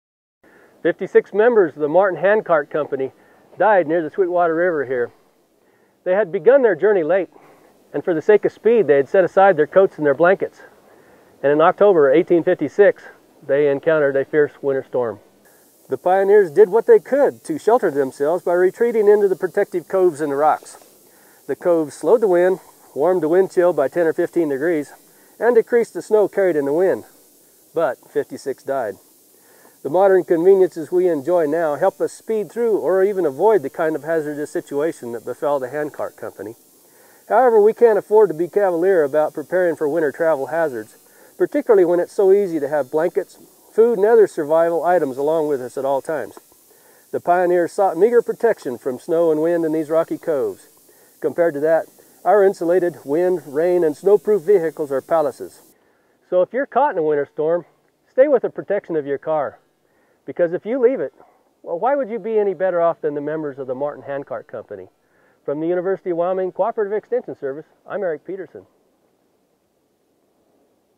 Martin's Cove
Leaving your car while traveling during a winter storm is dangerous, just as it was dangerous for pioneers to travel across Wyoming in the winter.